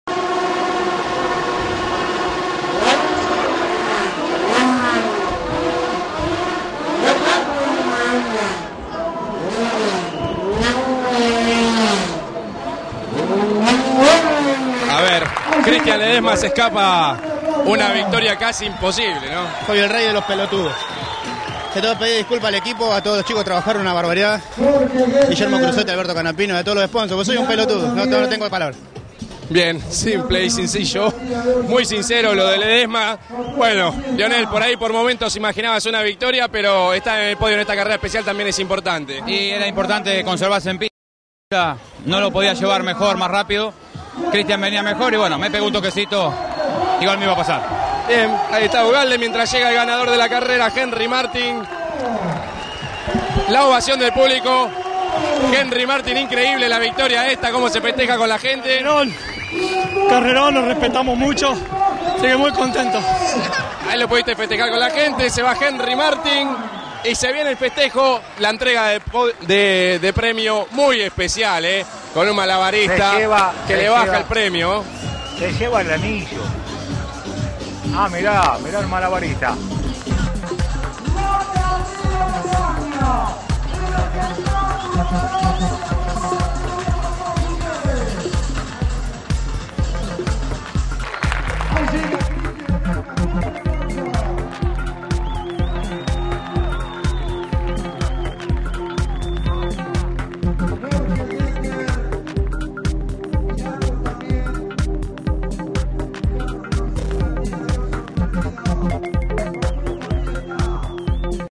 Top Race V6. El podio de la carrera del año 2008.
El podio de la carrera ganada por Henry Martin, seguido de Lionel Ugalde y Christian Ledesma.